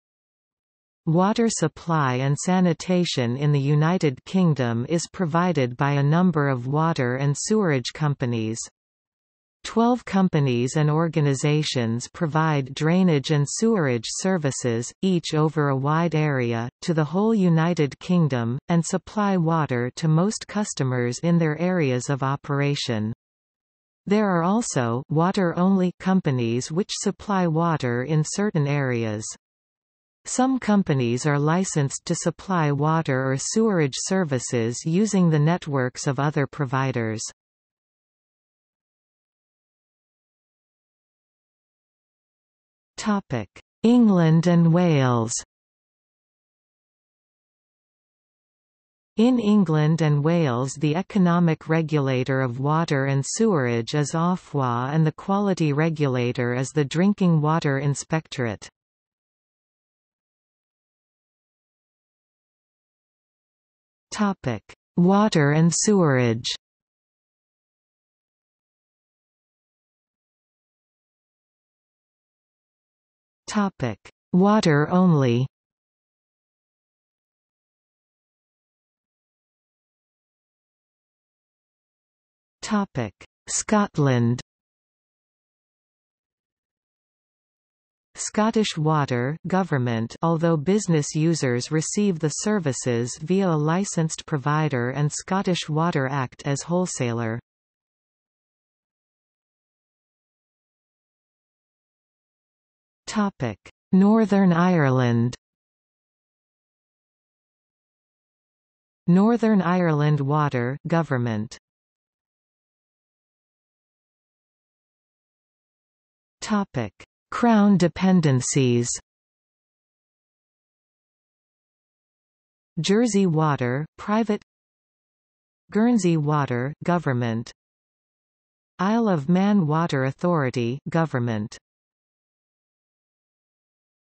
United Kingdom water companies | Wikipedia audio article
00:00:39 1 England and Wales
00:01:00 1.1 Water and sewerage
00:01:10 1.2 Water only
00:01:19 2 Scotland
00:01:39 3 Northern Ireland
00:01:52 4 Crown dependencies

- Socrates

SUMMARY
Water supply and sanitation in the United Kingdom is provided by a number of water and sewerage companies. Twelve companies and organisations provide drainage and sewerage services, each over a wide area, to the whole United Kingdom; and supply water to most customers in their areas of operation. There are also 'water only' companies which supply water in certain areas. Some companies are licensed to supply water or sewerage services using the networks of other providers.